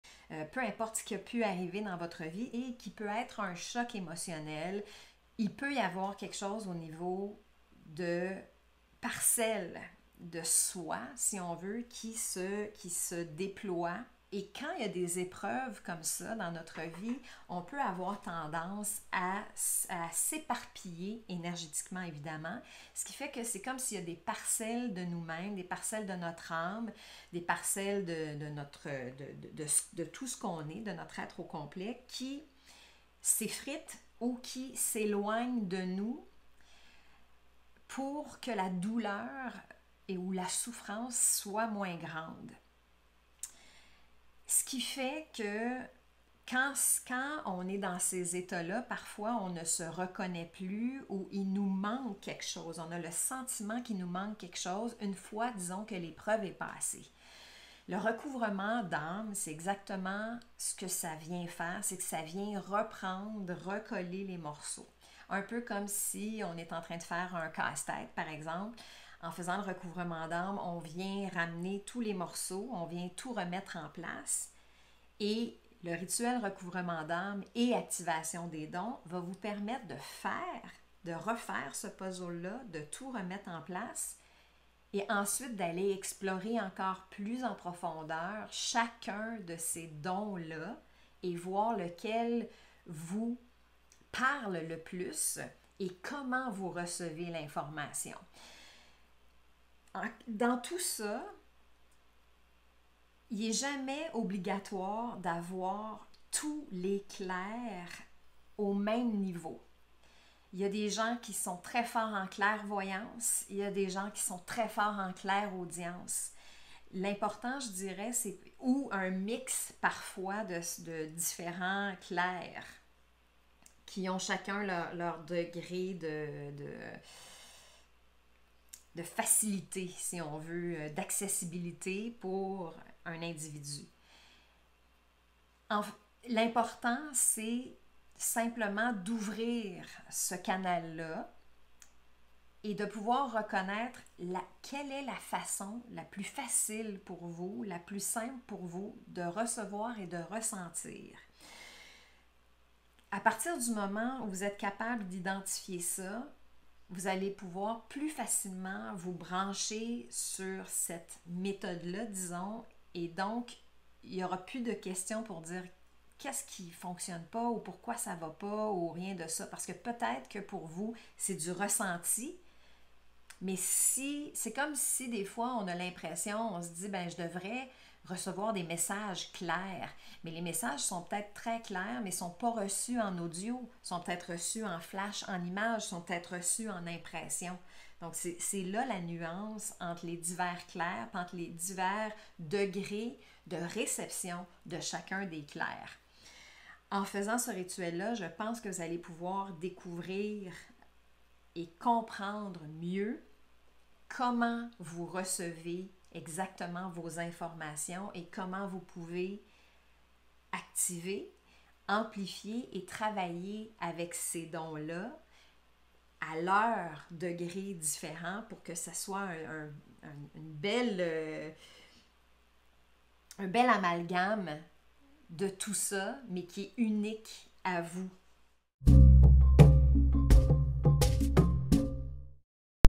Sentiment d'être incomplet.e et de ne pas avoir accès à vos dons? - CHAMANE URBAINE
Si vous avez le sentiment que vous n'êtes pas à la bonne place, que vous avez des ressentis sans savoir comment y accéder, je vous en parle ici.

Si vous avez des questions, écrivez-moi en commentaires!

Nathalie 🌿

Passez me dire Bonjour par ici! 💕